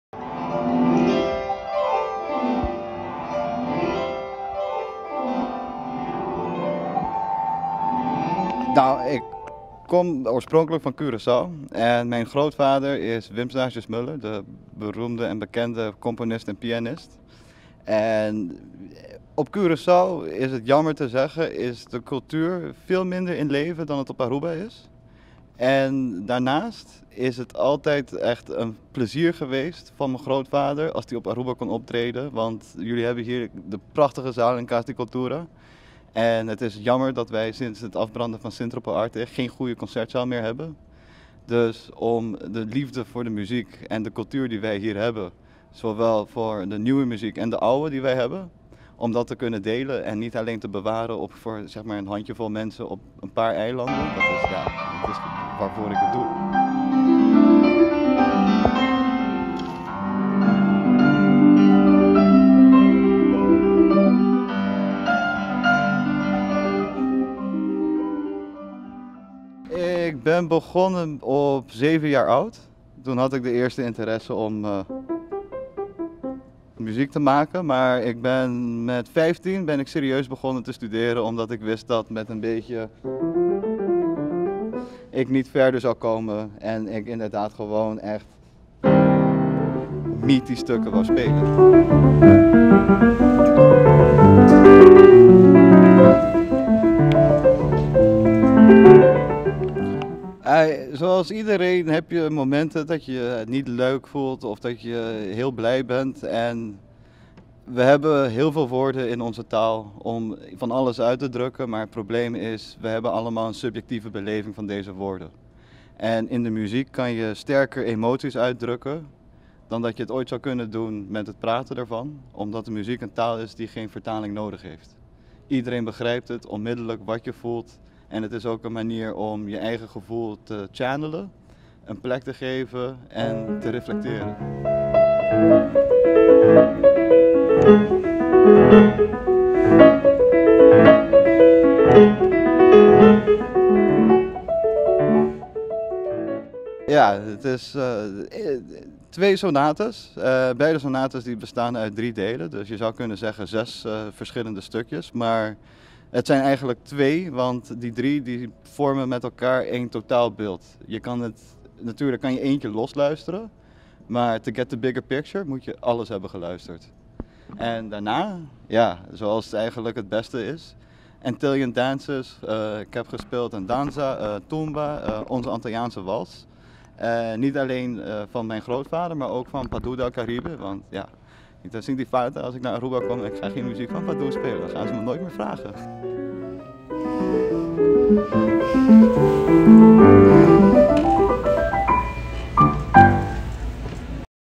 Alexander Kraft van Ermel cu concierto ‘De passie van de componist’
Diabierna a tuma luga e concierto di musica clasico De passie van de componist, unda cu por a scucha talento. A tuma e momento aki pa toca diferente cantica di Beethoven.

Alexander Kraft van Ermel, pianista, ta bini di Corsou y su welo ta Wim Statius Muller, kende ta un pianista famoso. E ta haya cu ta di lamenta cu na Corsou e cultura ta menos cu na Aruba. Semper tabata un placer pa ora su welo por a toca na Aruba. El a indica cu e sala di Cas di Cultura ta uno bunita y ta di lamenta cu desde Centro pa Arte a kima no tin un bon concierto. Nan no por comparti e amor pa musica y e cultura cu nan tin tanto pa cantica nobo como bieu, y no solamente warda pa un par di hende riba algun isla.